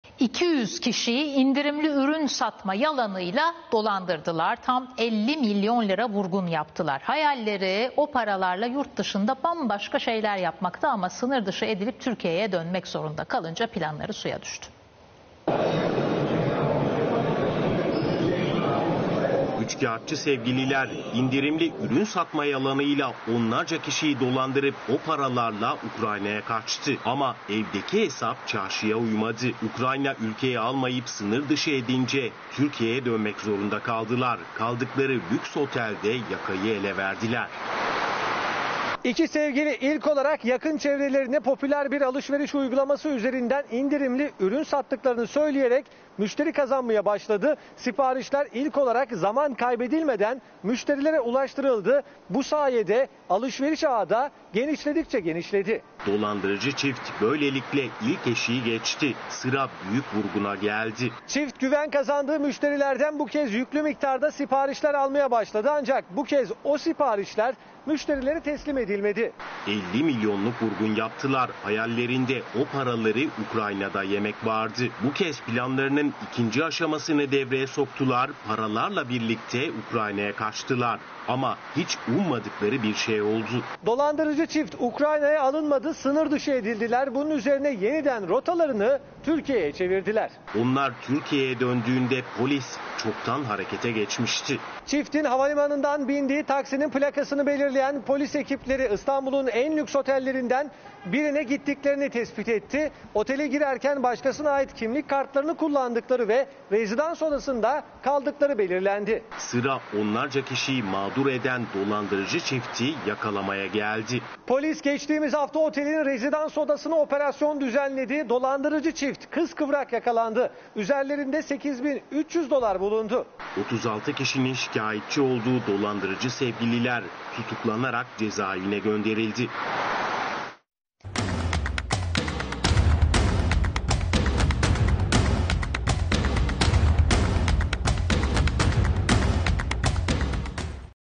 50 milyon liralık dev vurgun!
TAM 50 MİLYON LİRA VURGUN YAPTILAR... HAYALLERİ O PARALARLA YURTDIŞINDA BAMBAŞKA ŞEYLER YAPMAKTI AMA SINIR DIŞI EDİLİP TÜRKİYE'YE DÖNMEK ZORUNDA KALINCA PLANLARI SUYA DÜŞTÜ... LÜKS BİR OTEL REZİDANSINDA YAKAYI ELE VERDİLER.